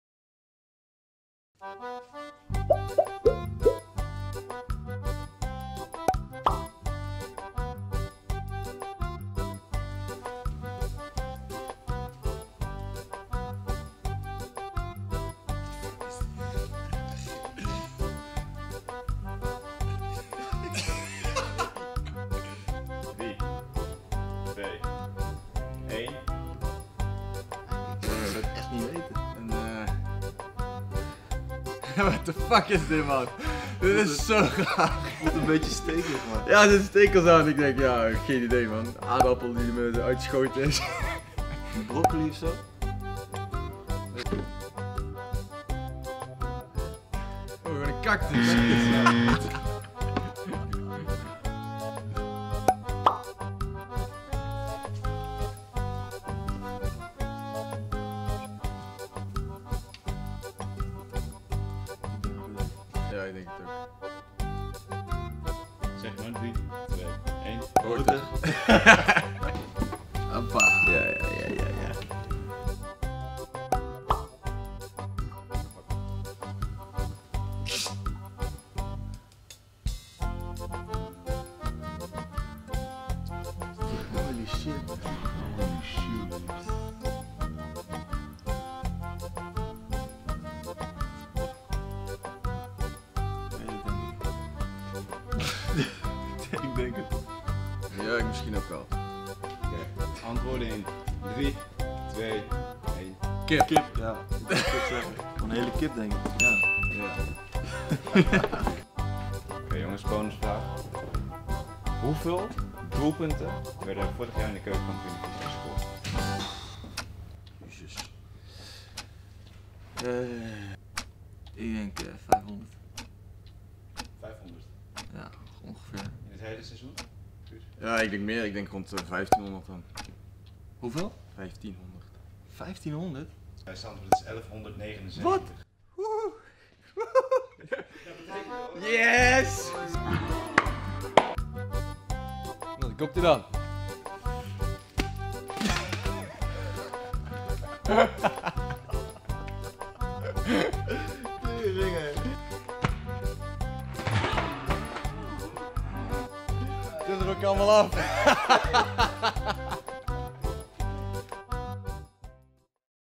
WHAT'S IN THE BOX? | Helmond Sport
Afl.1 - Helmond Sport

In de serie 'What's in the Box?' nemen duo's van verschillende Keuken Kampioen Divisie-ploegen het tegen elkaar op in de 'What's in the Box?-Challenge'. Er worden drie voorwerpen in een aquarium gelegd en de spelers raden welk voorwerp in het water ligt. De verliezer krijgt een passende straf.